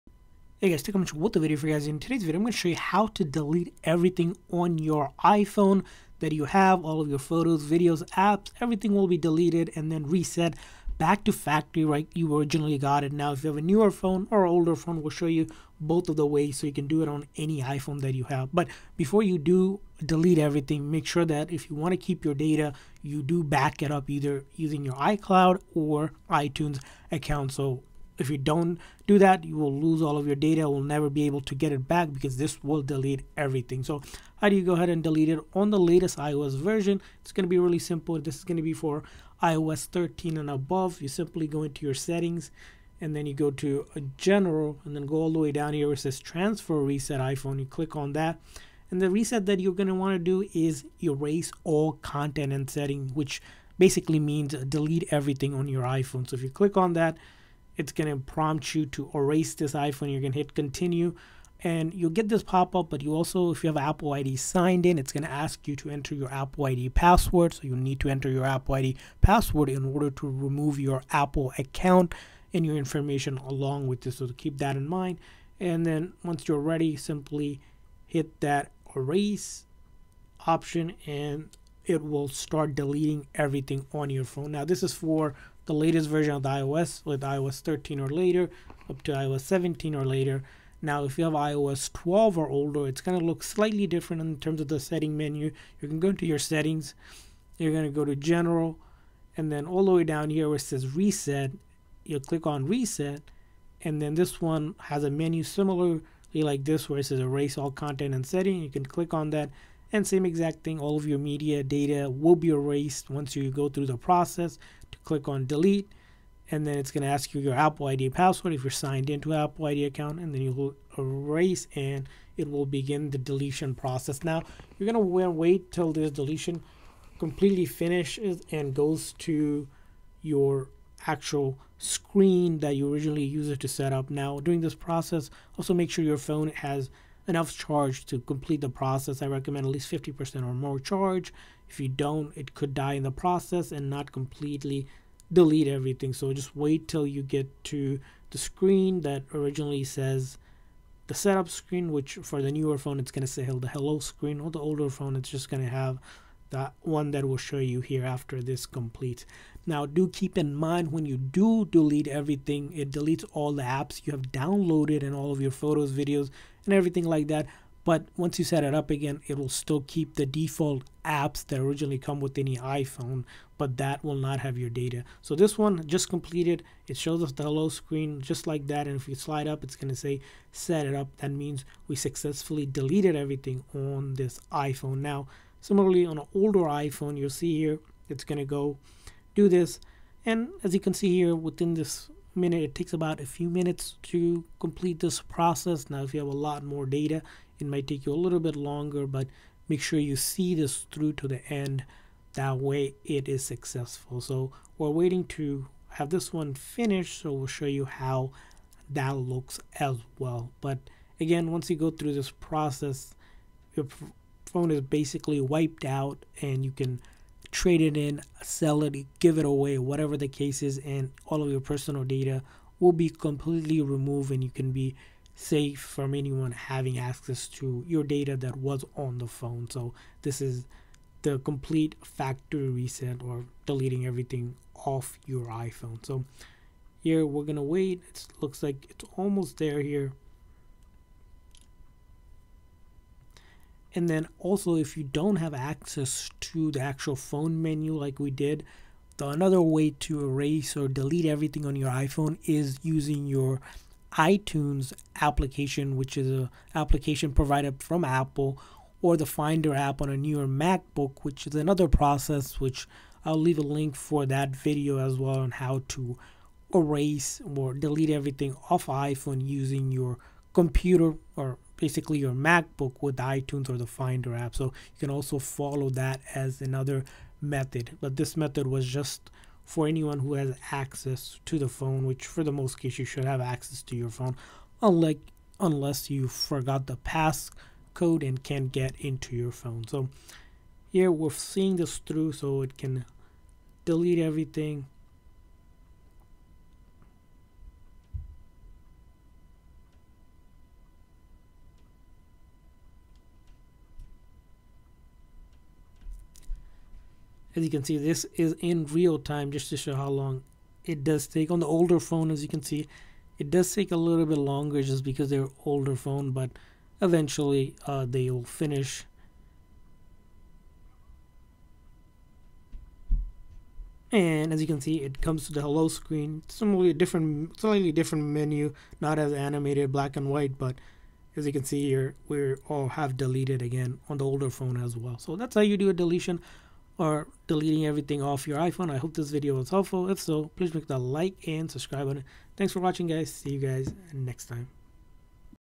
How to Delete Everything on Your iPhone
Need to delete all your stuff from your iPhone? If so you have come to the right place, in this video we teach you how to delete everything on your iPhone and reset it back to factory default settings.